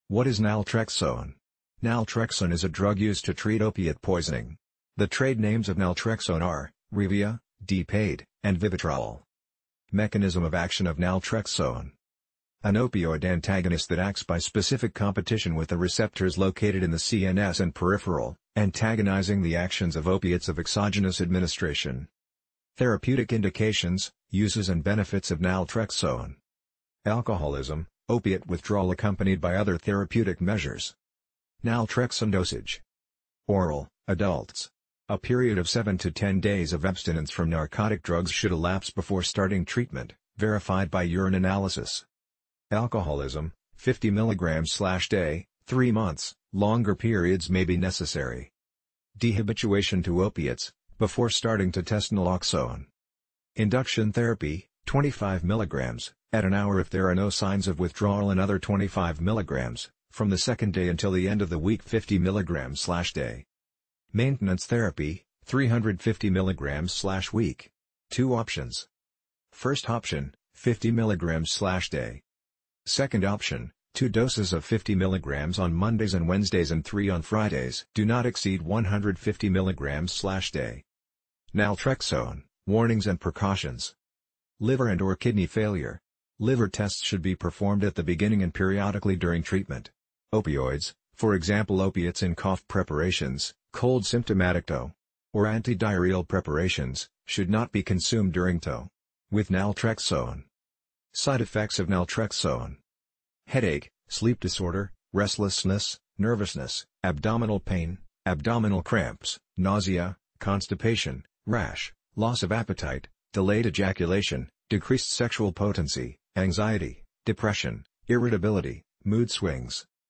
💊 What is NALTREXONE?. Benefits, uses, doses and side effects of naltrexone 50 mg (Revia/Vivitrol).
✅ Review about naltrexone 50 mg : Side effects, uses, doses, warnings, precautions, mechanism of action, contraindications, trade name (revia, vivitrol) and answers to questions like: what is naltrexone?, What is naltrexone used for?.
What is naltrexone?
Naltrexone is a drug used to treat opiate poisoning.
The trade names of naltrexone are: Revia, Depade and Vivitrol.